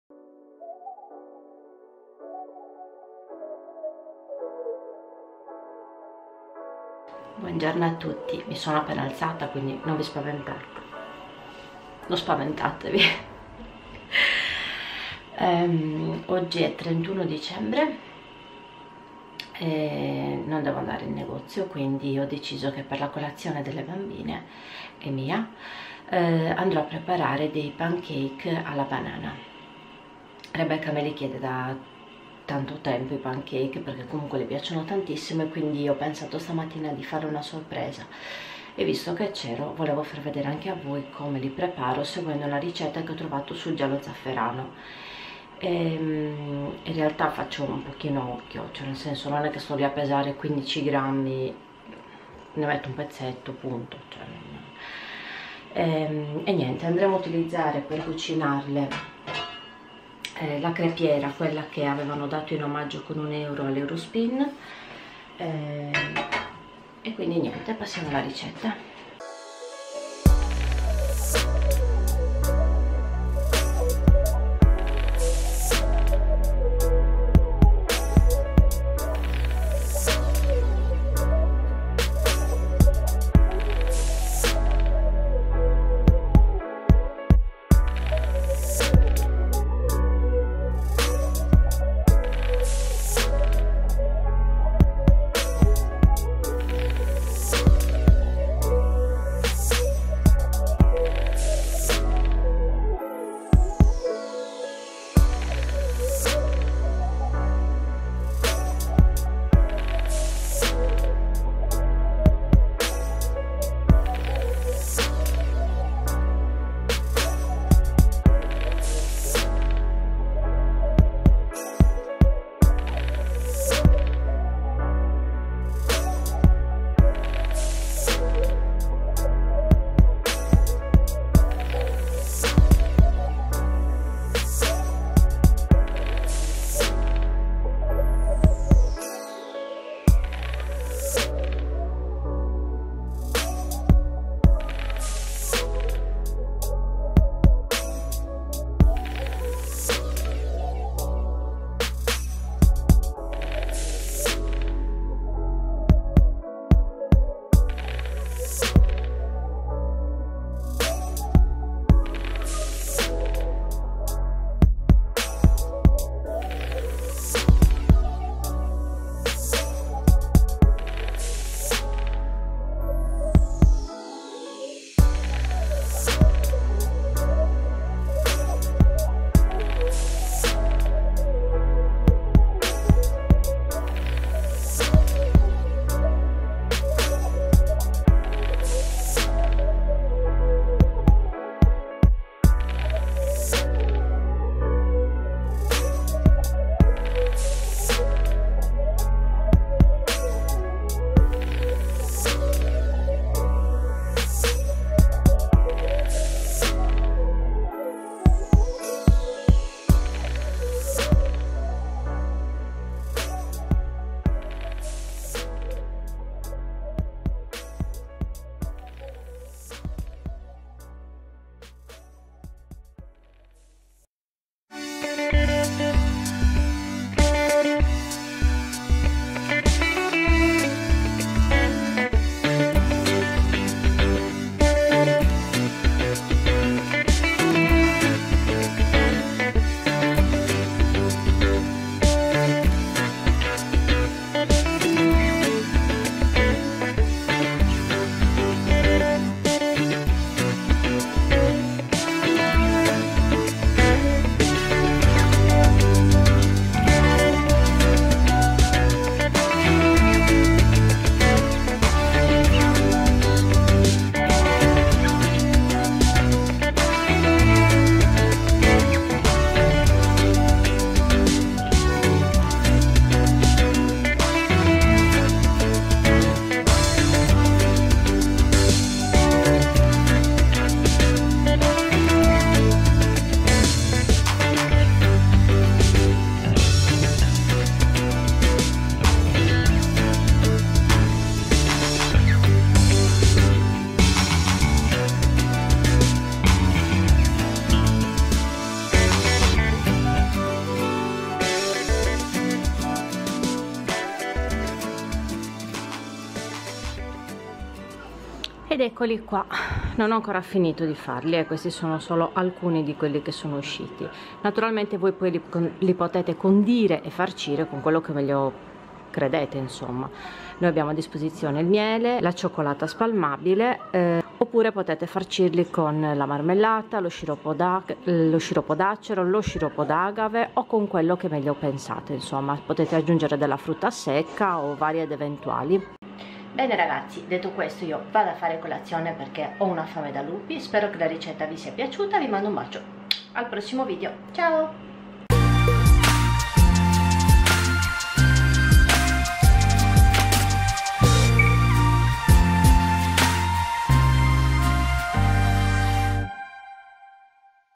Pancake alla Banana 🍌 ... facili e veloci ...
Scusate la faccia 😂😂😂 ma ero davvero stravolta!
Nonostante questo ho trovato il coraggio di riprendermi per condividere con voi anche questa ricettina!
Spero vi piaccia, un bacione.